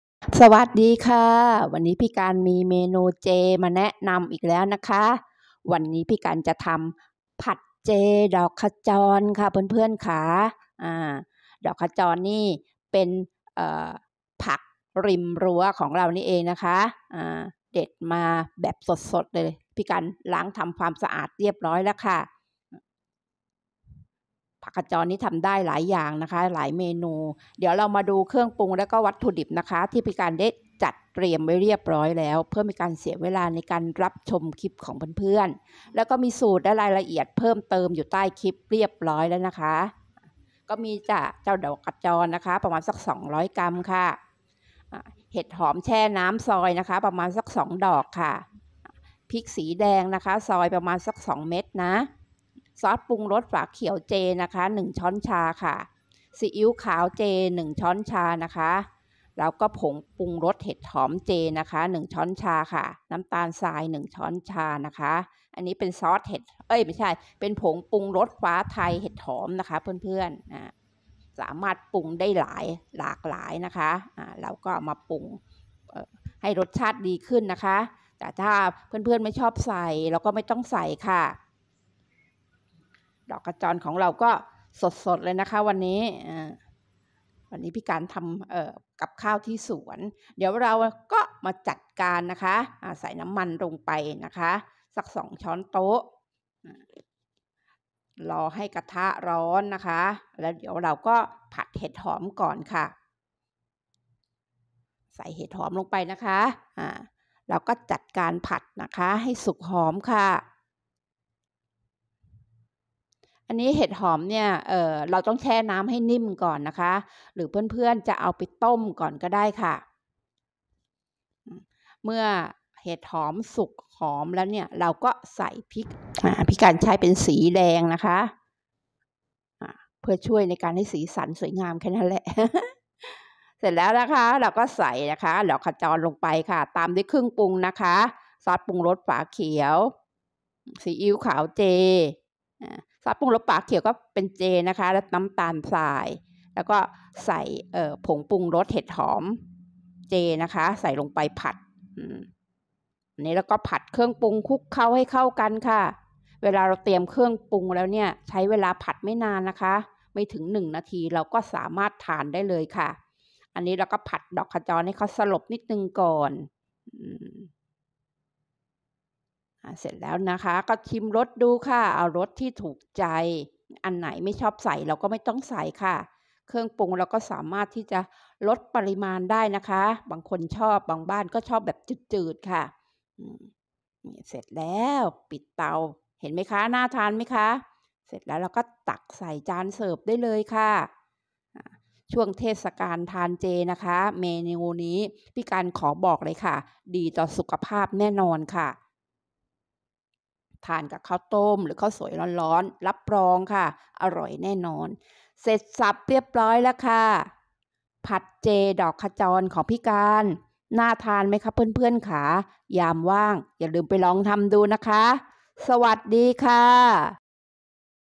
ผัดเจดอกขจร ผักข้างรั้วประโยชน์มากมาย อร่อยเลิศ ไม่ต้องเสียตังค์ แถมได้บุญด้วย พร้อมสูตรวิธีทำ
ผัดเจดอกขจร

วัตถุดิบและเครื่องปรุง

    ดอกขจร              200    กรัม
    เห็ดหอมซอย           2    ดอก
    พริกอ่อนสีแดงซอย  2   เม็ด
     ซอสปรุงรสชาเขียว  1   ช้อนชา
     ผงปรุงรสเห็ดหอม     1   ช้อนชา